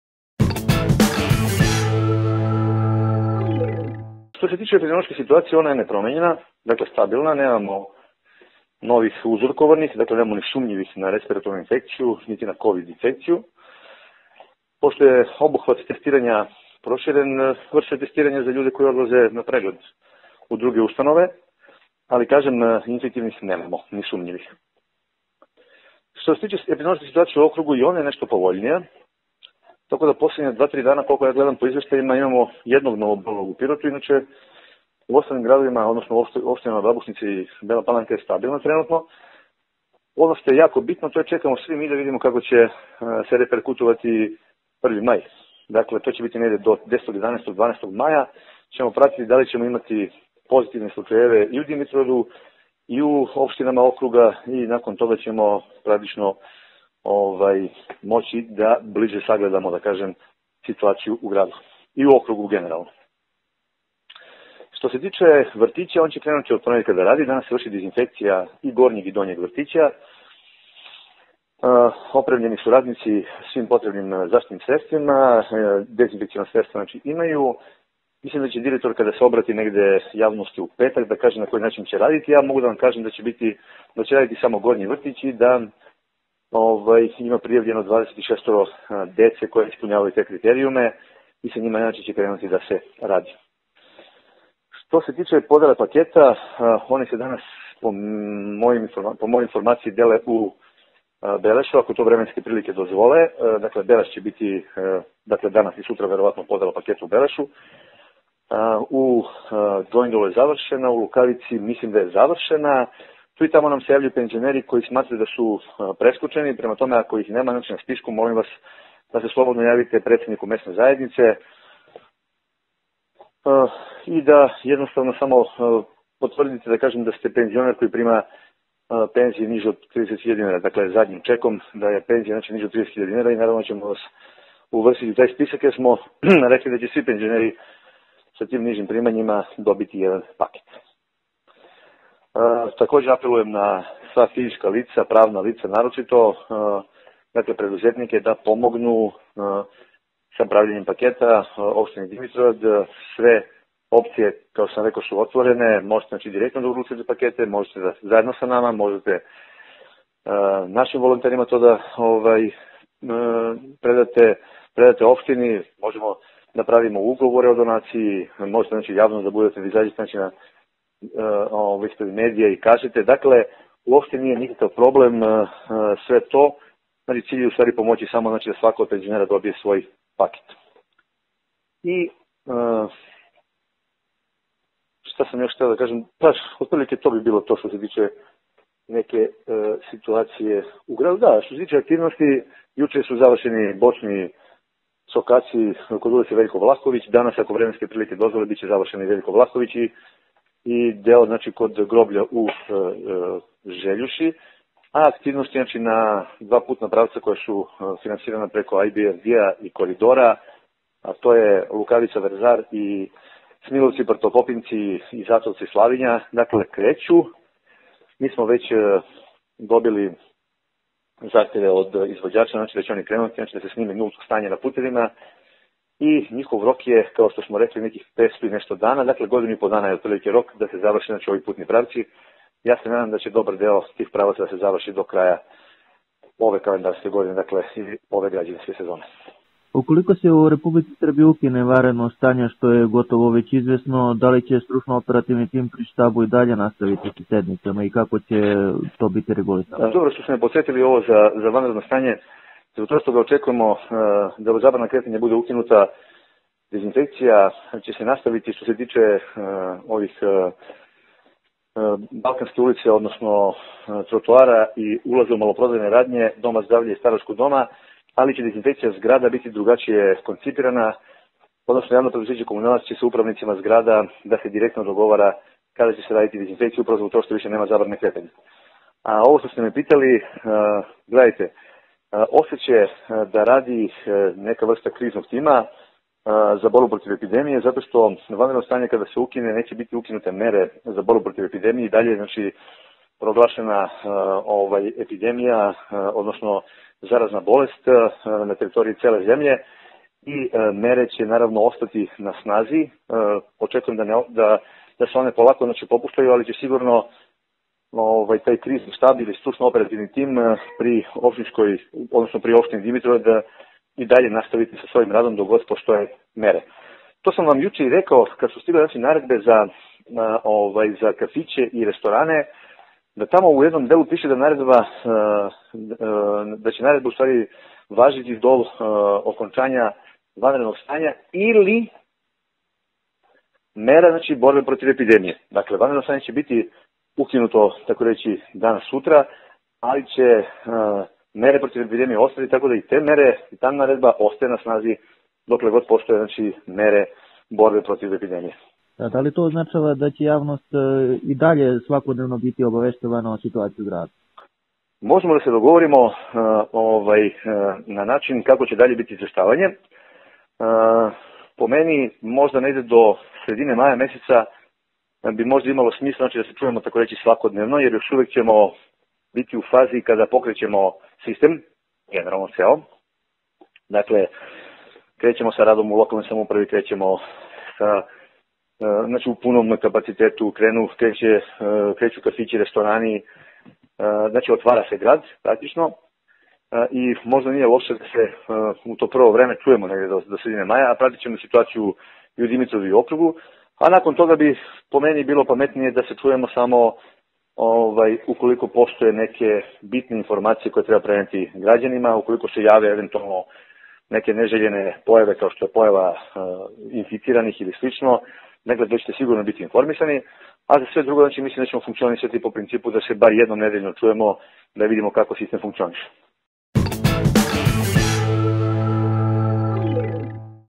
Dimitrov: Epidemiološka situacija u Dimitrovgradu stabilna, od ponedeljka počinju da rade vrtići
Epidemiološka situacija u Dimitrovgradu je nepromenjena i stabilna, nema novih uzorkovanih niti sumnjivih na respiratorne infekcije i na Covid-19. Ovo je u telefonskom razgovoru za FAR potvrdio Vladica Dimitrov, komandant Opštinskog štaba za vanredne stuacije.

Dimitrov je naglasio da je situacija i u Pirotskom okrugu nešto povoljnija.

Vrtići počinju da rade od ponedeljka, a danas se vrši dezinfekcija u oba objekta u gradu, kaže komandant štaba.

Prema njegovim rečima, podela paketa hrane i higijene penzionerima sa primanjima manjim od 30.000 dinara, u zavisnosti od vremenskih uslova, danas i sutra, vrši se u Belešu, dok je u Gojin Dolu i Lukavici završena.

On poziva penzionere koji nisu bili na spisku, a ispunjavaju uslove za pomoć da se jave predsednicima Mesnih zajednica nakon čega će biti uvršteni u spisak.

Na pitanje FAR-a da li će Opštinski štab zasedati i nakon ukidanja vanrednog stanja u Srbiji, Dimitrov je kazao da će stručno-operativni tim pri Štabu i dalje nastaviti sa svojim radom dok još budu postojale mere borbe protiv epidemije koronavirusa.

Više detalja u izjavi Vladice Dimitrova.

Novinar: Aleksandar Todorov
Montaža: Mikica Kostov
Odgovorni urednik: Petar Videnov
Foto: Stefan Pavić
Produkcija: FAR TV
maj 2020